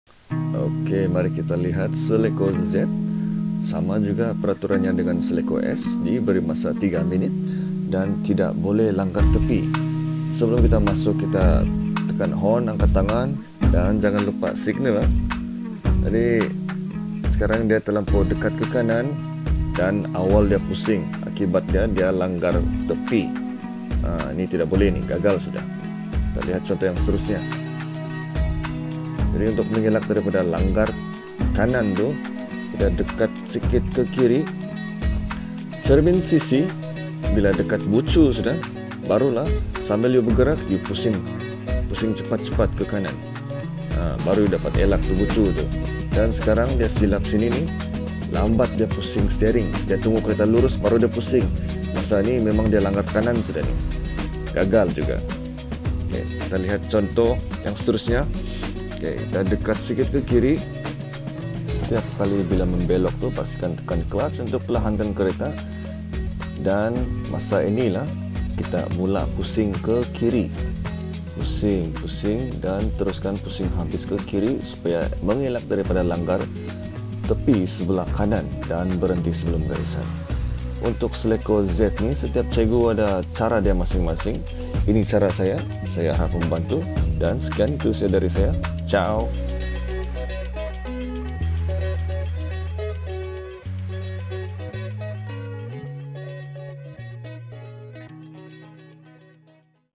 Selekoh Z |Pandangan Atas
Menunjukkan kesalahan-kesalahan yang sering dilakukan oleh pelajar semasa latihan dan ujian serta cara bagaimana untuk mengelak dari melanggar tepi. Harap rujukan ini cukup jelas dan membantu.

Kesalahan-kesalahan MANDATORI seperti berikut;

1. Melanggar atau menyentuh benteng dan garisan.
2. Tidak berhenti di simpang.
3. Melebihi masa 3 minit.
4. Terlibat dalam kemalangan.

VIDEO - VIDEO LAIN:-